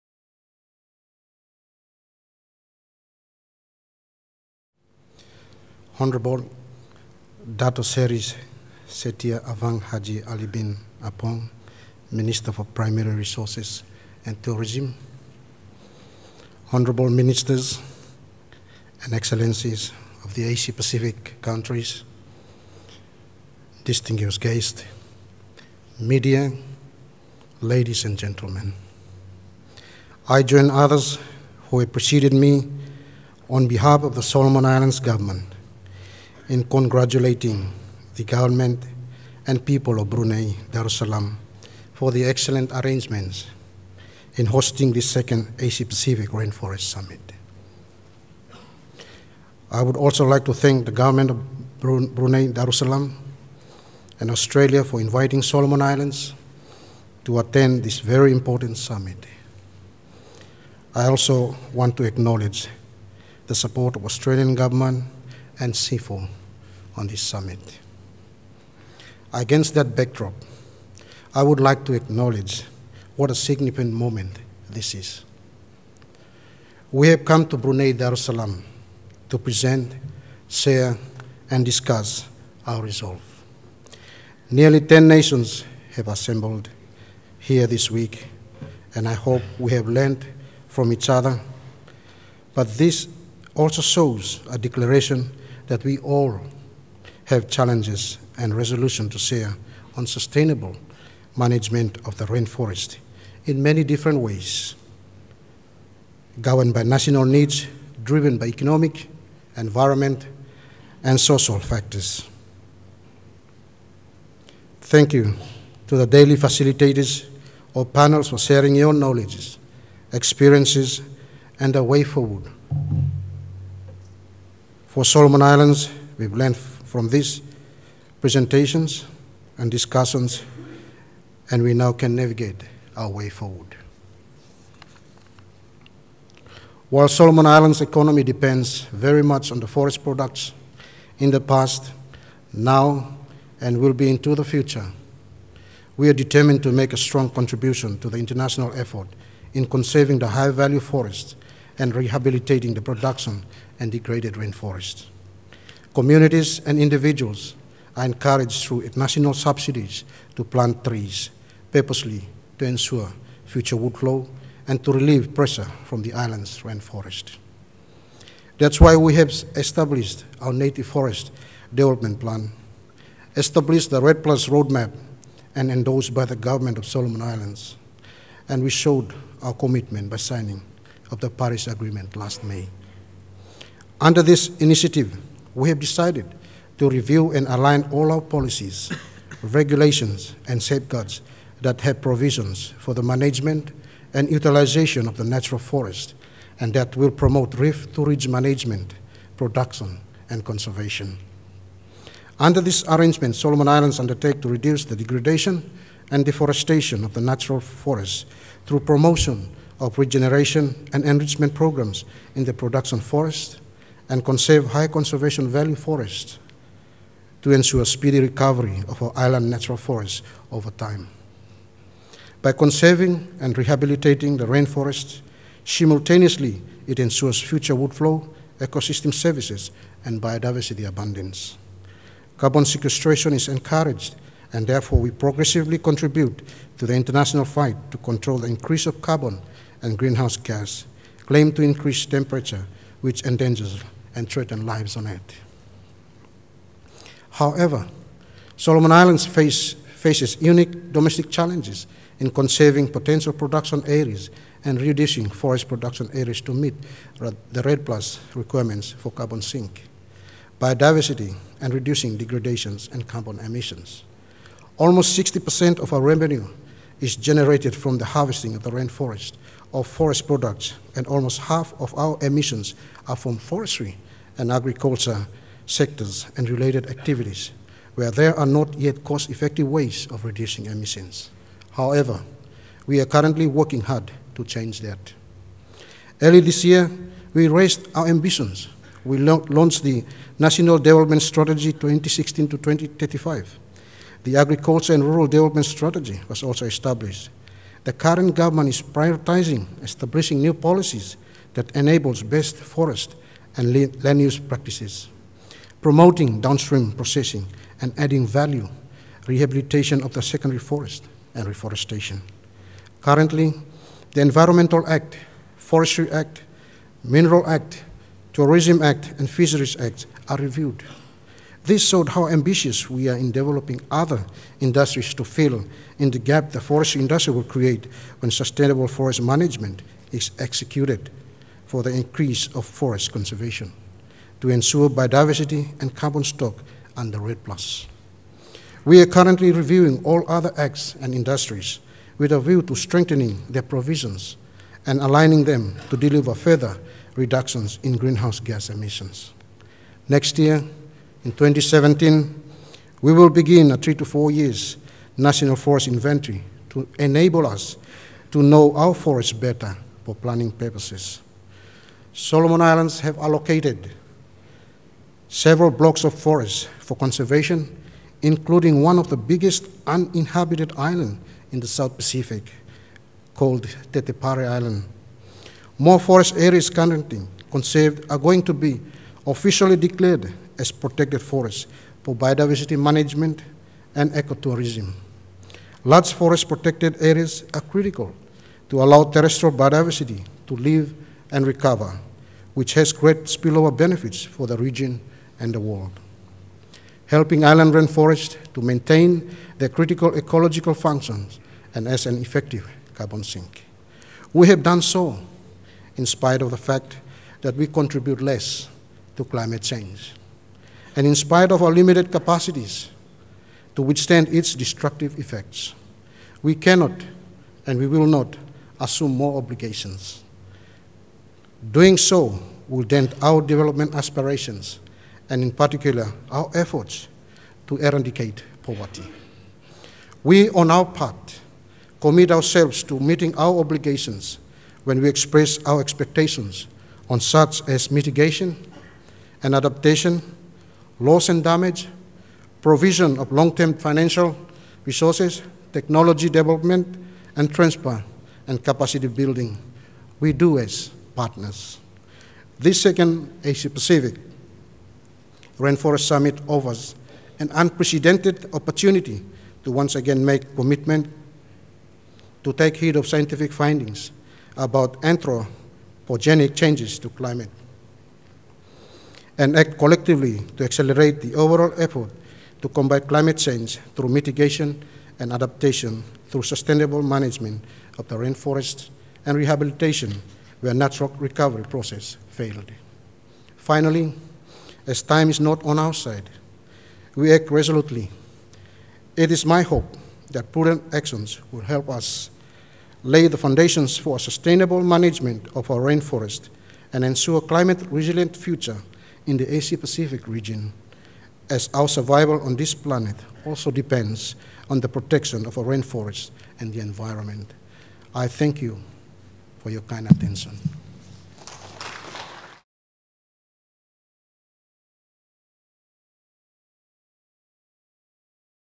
Solomon Islands: Committed to conserving rainforests
HE Vaeno Vigulu, Permanent Secretary at the Solomon Islands Ministry of Forestry and Research, delivers an address on 4 August 2016 at the Asia-Pacific Rainforest Summit in Bandar Seri Begawan, Brunei Darussalam.

Leaders in public, private and community sectors from across the Asia-Pacific gathered to discuss the future of the region’s forests at the Summit held from 3-5 August.